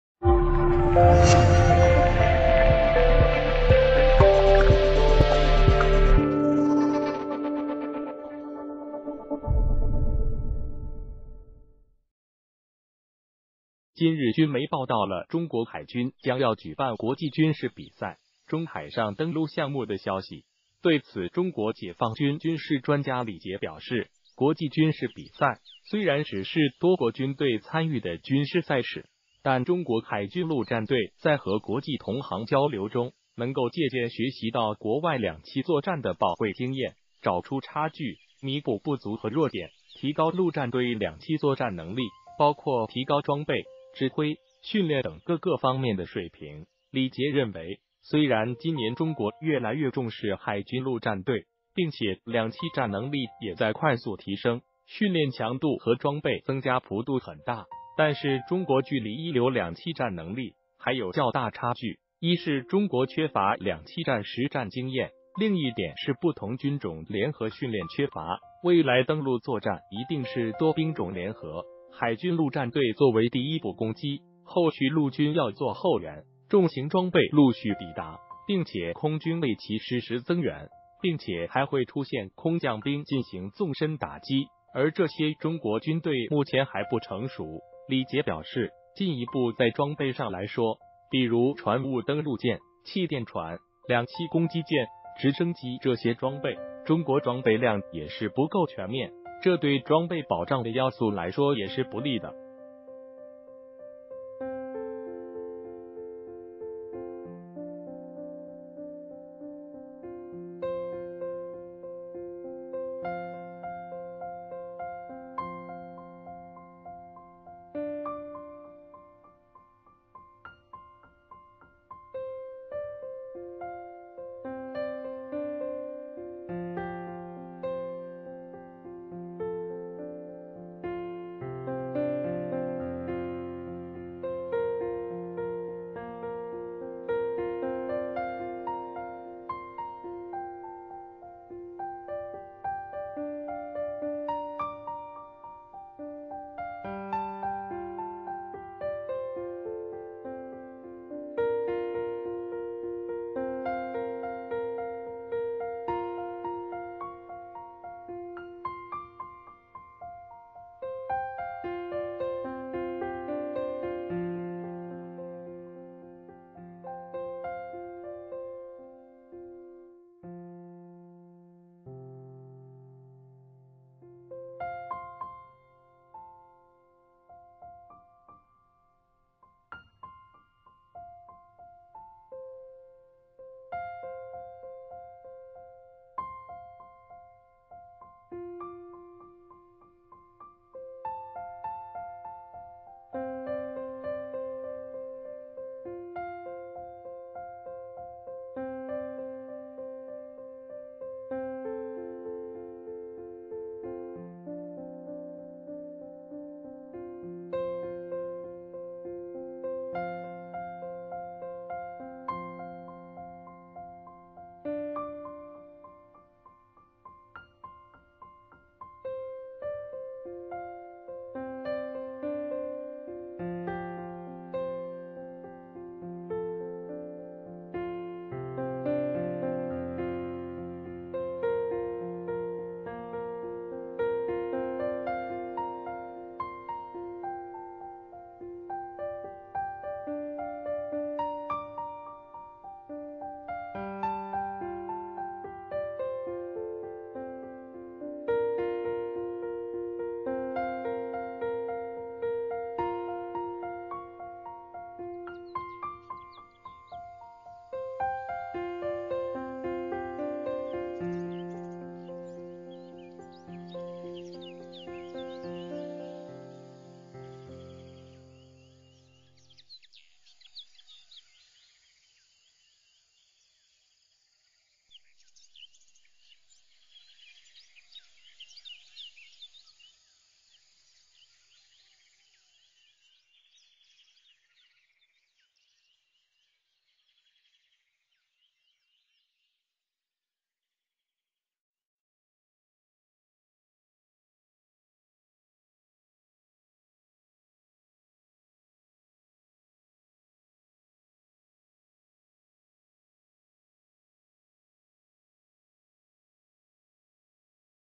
解放军专家直言，解放军重视两栖作战，但是仍没达到一流水平！
谢谢！解放军专家直言，解放军重视两栖作战，但是仍没达到一流水平！